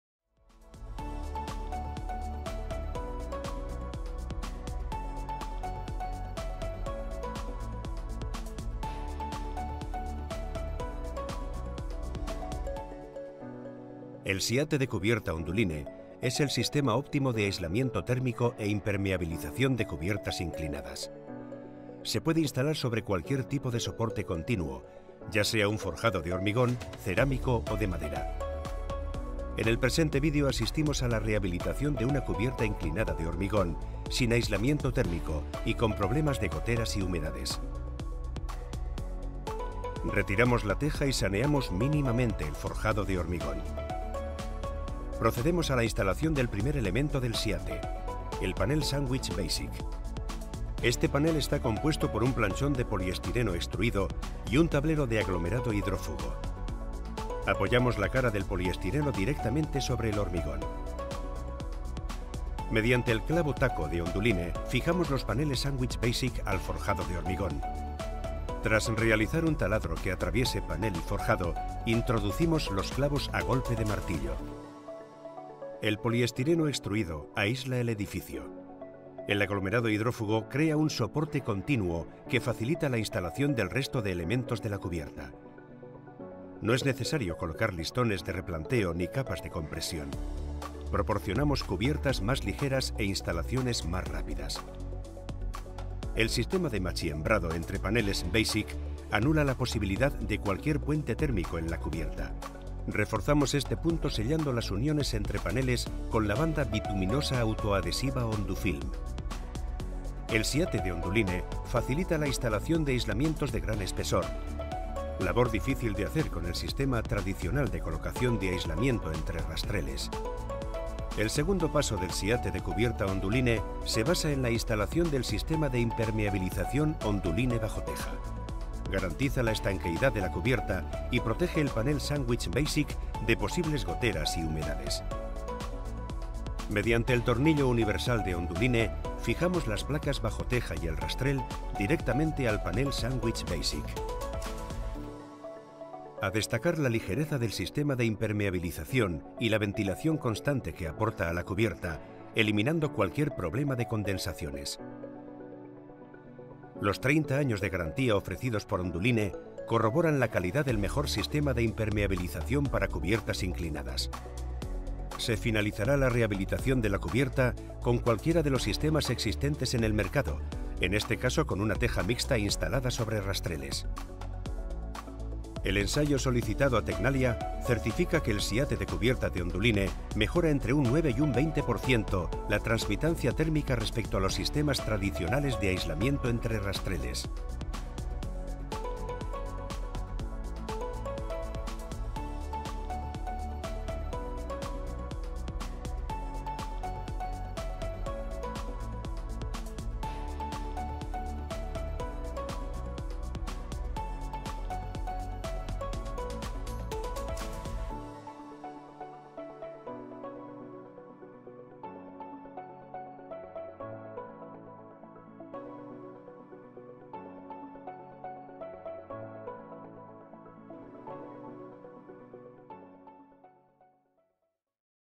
Rehabilitación, impermeabilización y aislamiento de tejados | BdB + Onduline
El Sistema SIATE de Cubierta de Online es una solución óptima para la rehabilitación energética de tejados y cubiertas inclinadas. Este sistema aporta de forma sencilla y rápida la impermeabilización y aislamiento del tejado.

En este video junto a Online, te enseñamos un caso real de una rehabilitación de una cubierta inclinada de hormigón, sin aislamiento térmico y con problemas de goteras y humedades.

Podrás ver:
☑️ Beneficios de la aplicación del Sistema SIATE de Onduline
☑️ Paso a paso y materiales
☑️ Consejos y tips útiles para la perfecta aplicación

Grupo BdB es tu tienda especialista en materiales de construcción. Contamos con los mejores productos y soluciones para obras, reformas o reparaciones, de marcas líderes como Onduline.